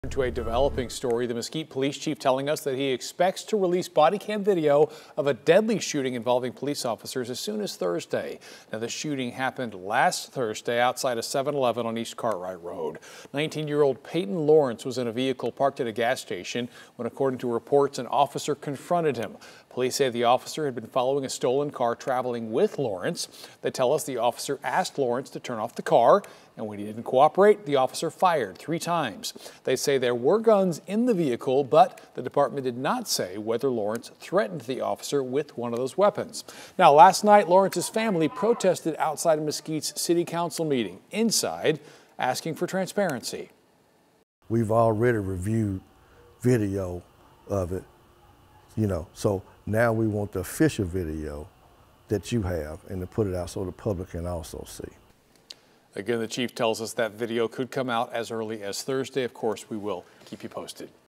Mesquite, TX: Police to release bodycam footage of officer shooting that killed 19-year-old
Officials say Payton Lawrence was fatally shot by police, three others were detained, and two more are on the run following an incident over a stolen car.